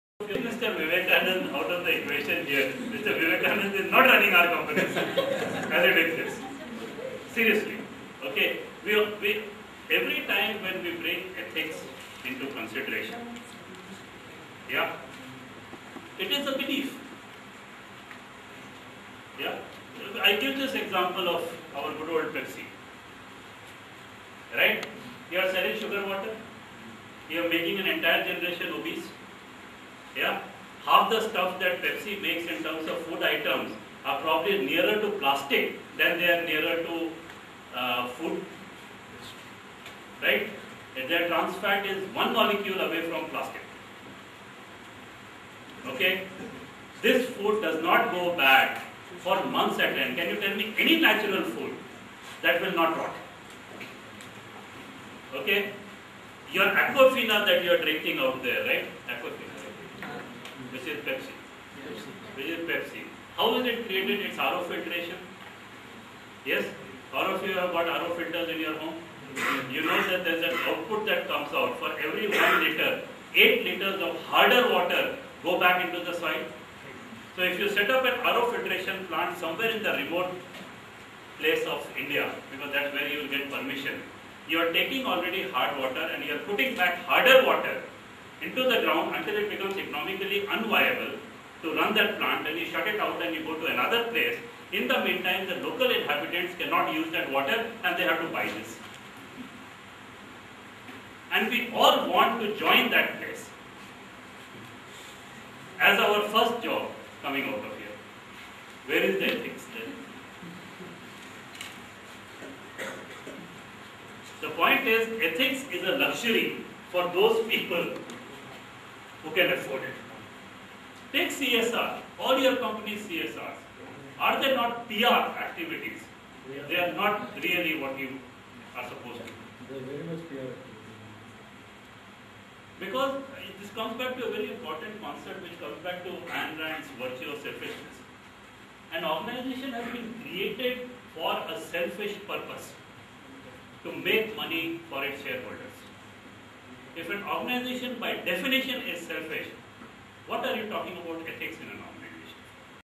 Ethics is a luxury
If you join a company and take its salary, can you afford the luxury of personal ethics that run contrary to the corporate culture?